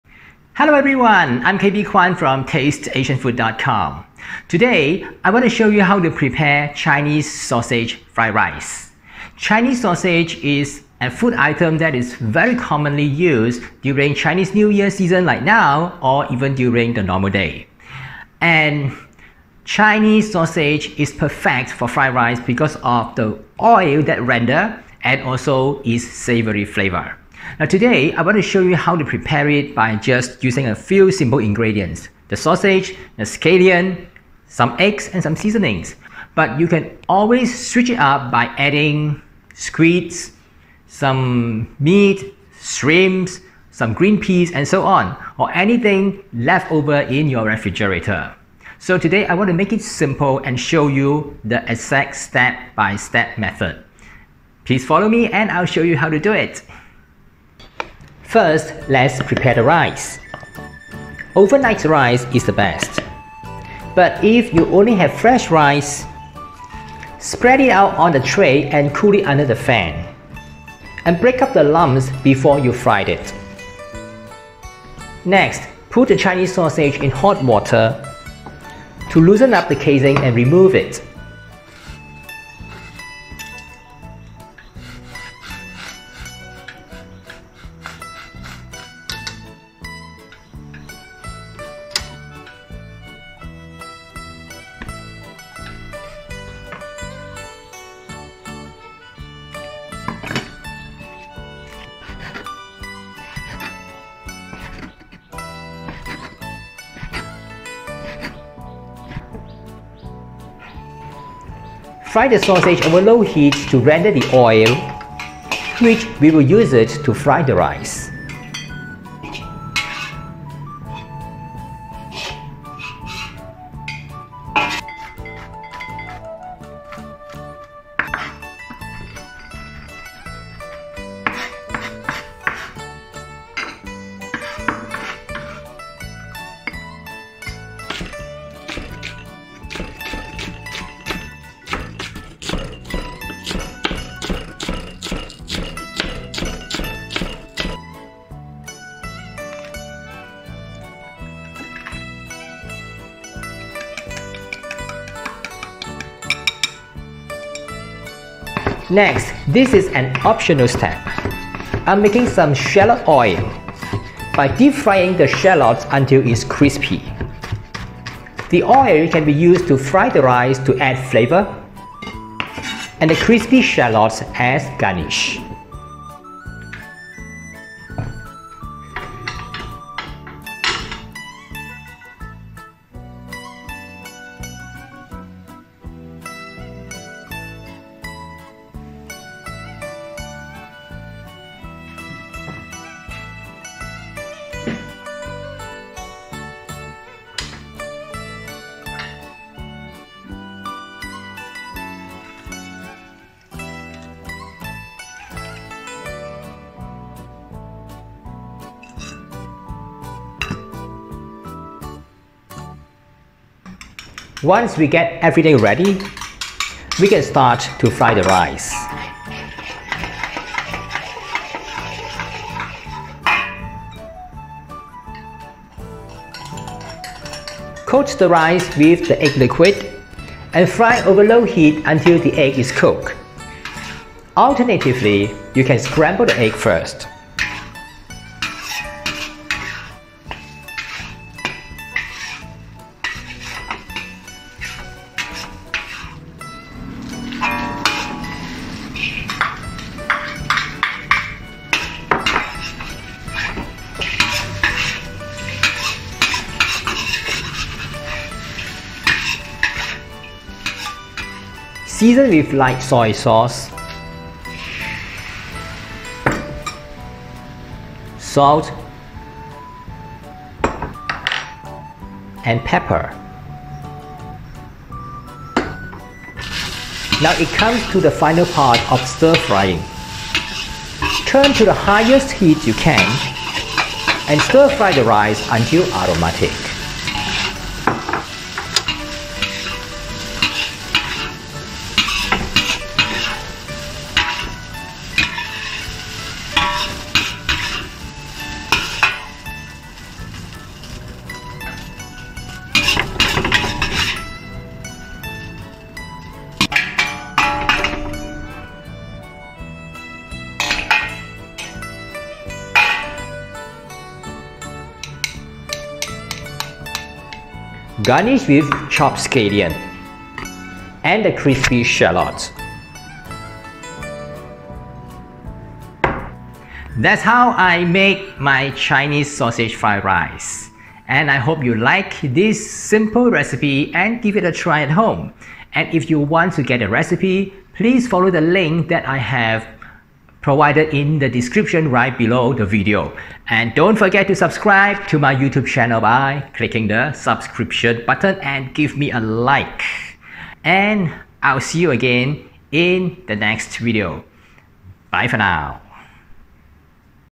Chinese sausage fried rice - how to prepare in 20 minutes
Chinese sausage fried rice 臘腸炒飯 is a quick and easy dish.  This recipe shows you all the steps in detail on how to cook it in 20 minutes.
This dish is also a welcome respite from the elaborate cuisine and binge eating during the Chinese New Year.
Therefore, I have made this simple fried rice with some Chinese sausage, and hope that this is the recipe you are looking for after preparing all the complicated Chinese New Year dishes.

Ingredients
2 tbsp cooking oil (or shallot oil)
2 cloves garlic, minced
1 Chinese sausage, cut into diagonally
2 eggs, lightly beaten
250g (9 oz) overnight, leftover steamed rice
2 tsps light soy sauce
1/2 tsp ground white pepper,
1/4 tsp salt
2 tbsp chopped scallion
2 tsp fried shallot

Instructions
- Break the lumps of cooked rice with your hand.
- Deep-fried the shallot until crispy. Use the shallot oil to fry the rice.
- Place the Chinese sausage in boiling water for twenty seconds. The casing will bulge out and detached. Remove it from the pot and let it cools.
- Cut the Chinese sausage into small dices.
- Stir-fried the Chinese sausage until the oil rendered.
- Remove the Chinese sausage and keep the oil in a wok. Add some shallot oil if it is not enough.
- Saute the chopped garlic until aromatic.
- Pour the beaten egg into the wok. Add the rice.
- Stir-fry it over low heat until the egg is fully cooked and encase each grain of rice.
- Seasoned rice with light soy sauce, salt, and ground white pepper.
- Now turn to high heat to stir-fried rice until aromatic.
- Add the scallions.
- Dish out immediately. Garnish with more chopped scallions and fry shallots.